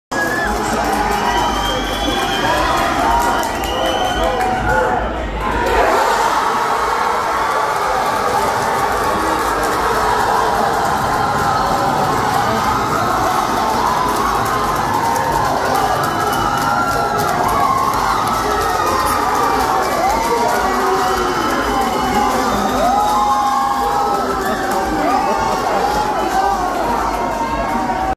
Rugby World Cup 2023: The Most Insane Moments
Watch the 2023 Rugby World Cup South Africa semi-finals at Hooters in this exciting video! Don't miss out on the action from this epic tournament.